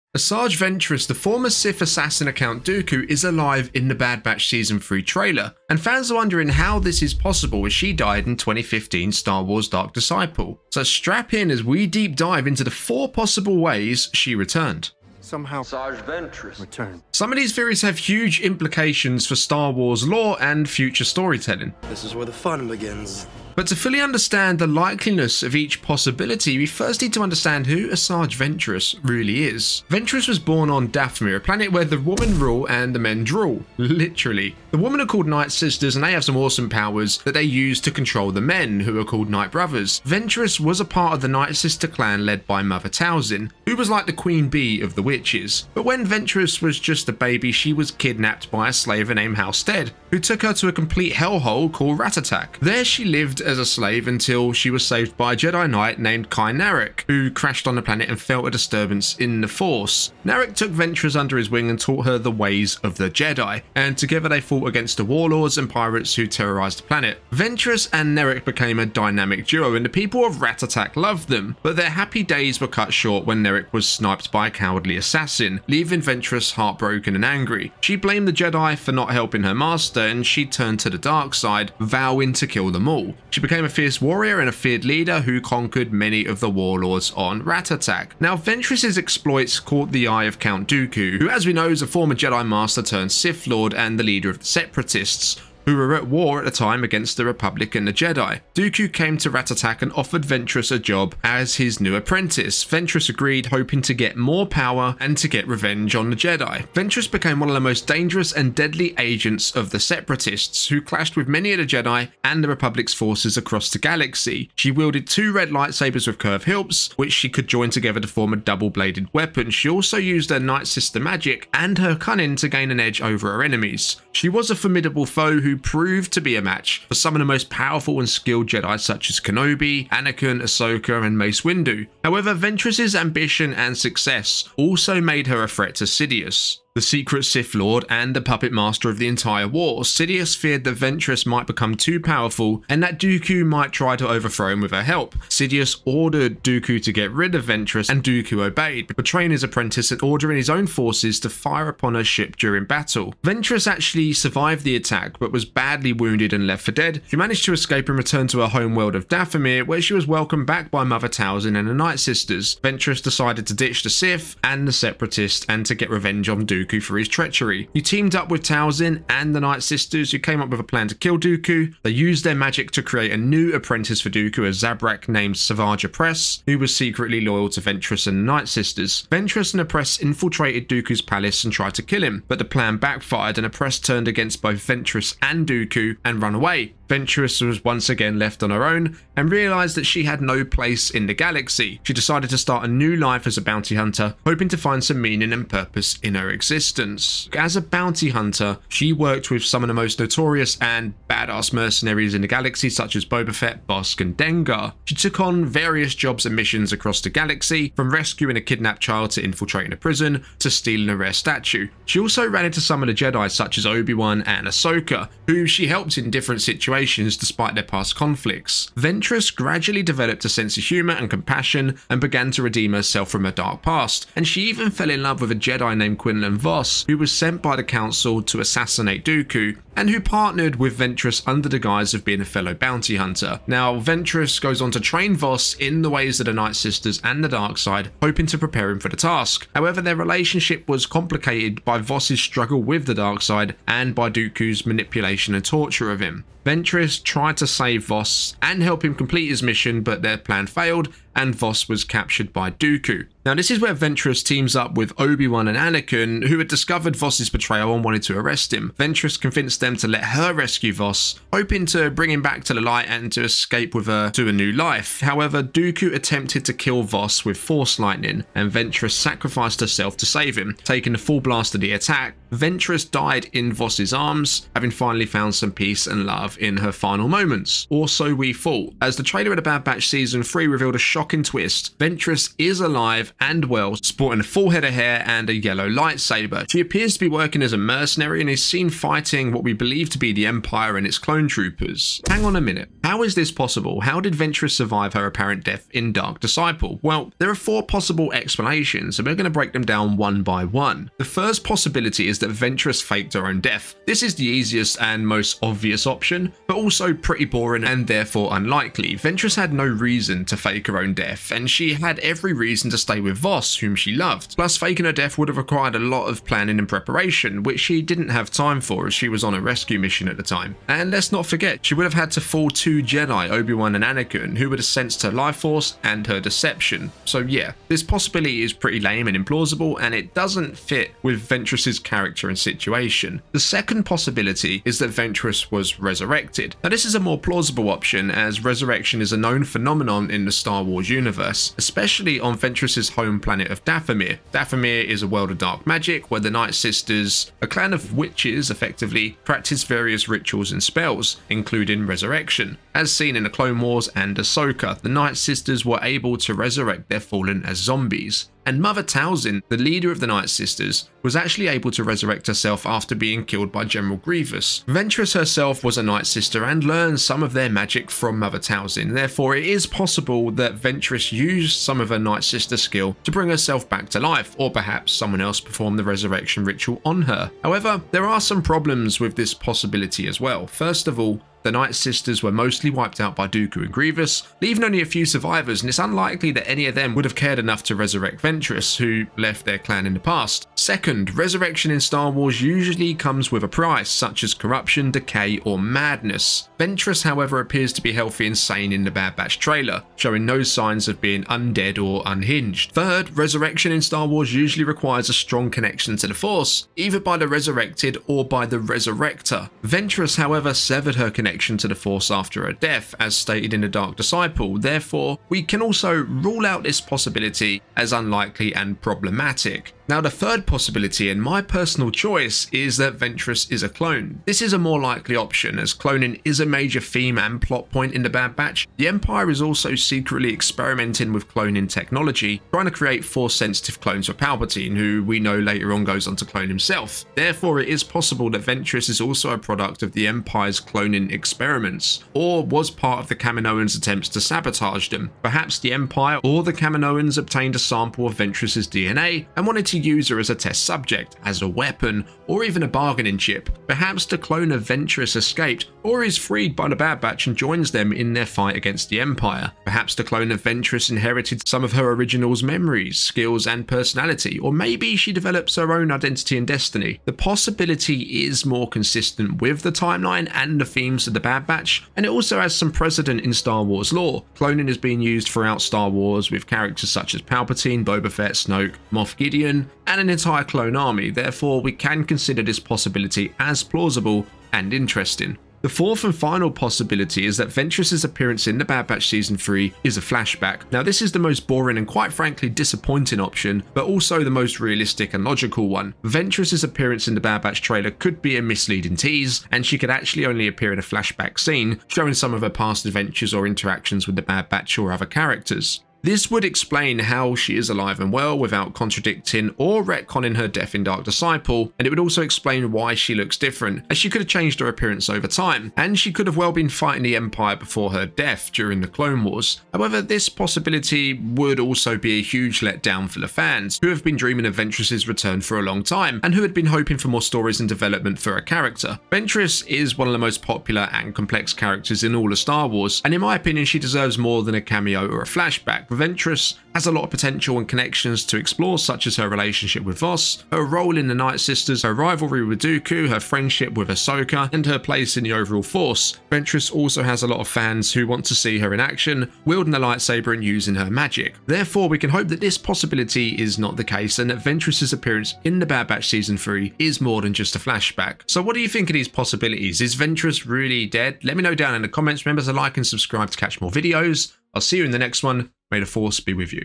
How Asajj Ventress Returns In The Bad Batch Season 3 - Theory
Asajj Ventress, the former Sith assassin and bounty hunter, was supposed to be dead after the events of Star Wars: Dark Disciple. But the trailer for The Bad Batch Season 3 reveals that she is alive and well, and has a mysterious connection to the elite clone squad. How did she survive, and what is her role in the final season of the Star Wars spin-off series? In this video, we will explore the 4 possible ways that Asajj Ventress returned from the dead, and what they mean for the future of the galaxy.